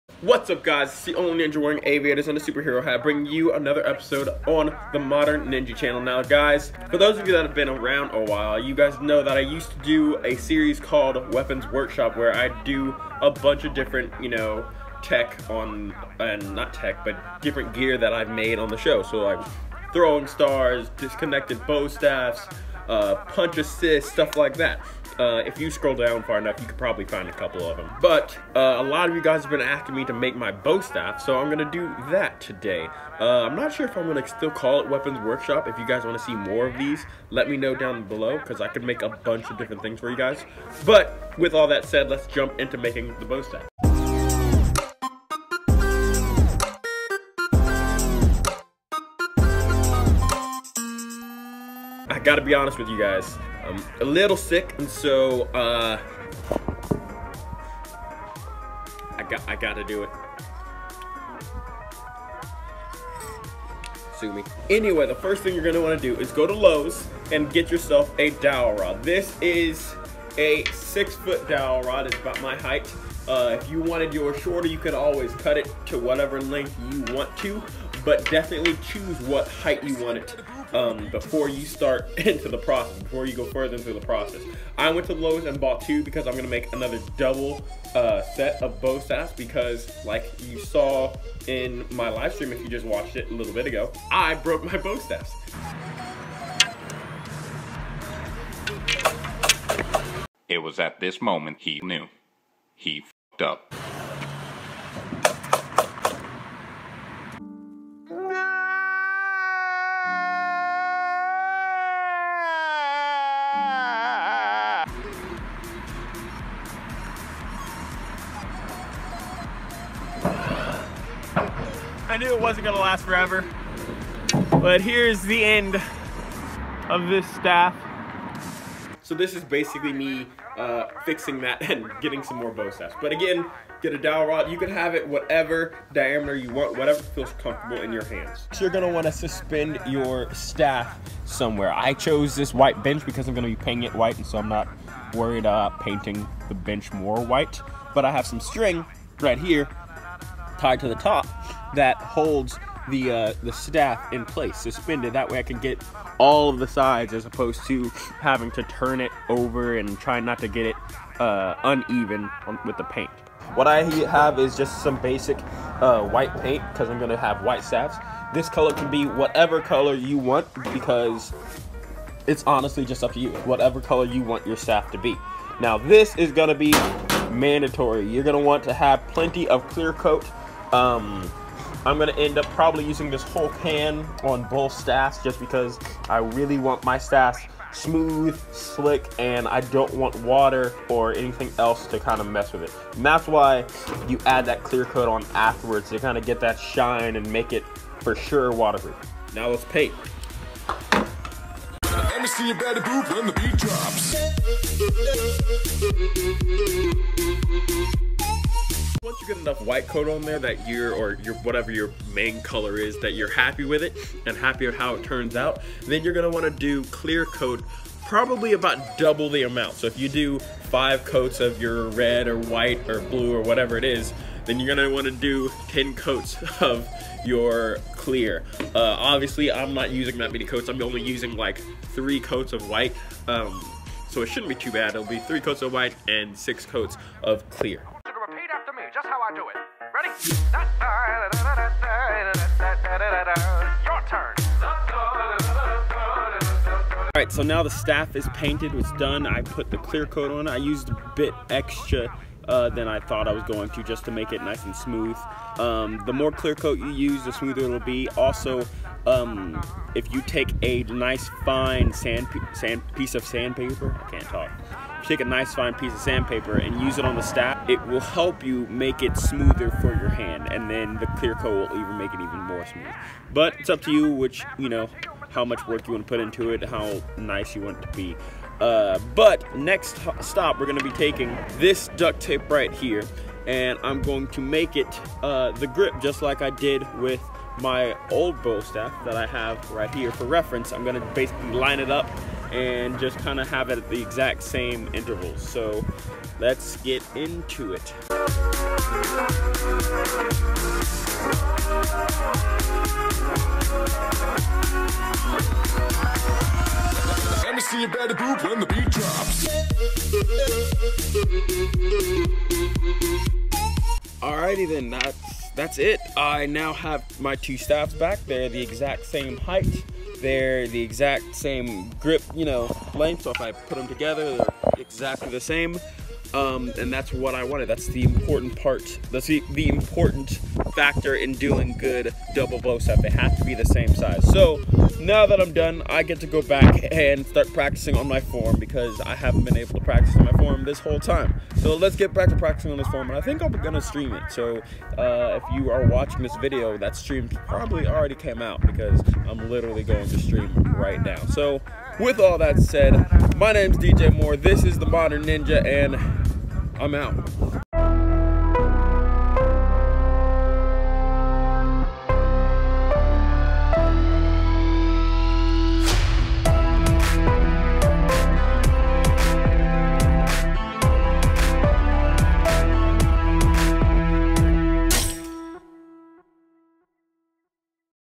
How to Make A $3 Bo Staff | Weapons Workshop
CHEAP Bo Staff, made by yours truly. You have been asking about this for a while now, so I better answer. Here is how to make my bo staff!

My Hat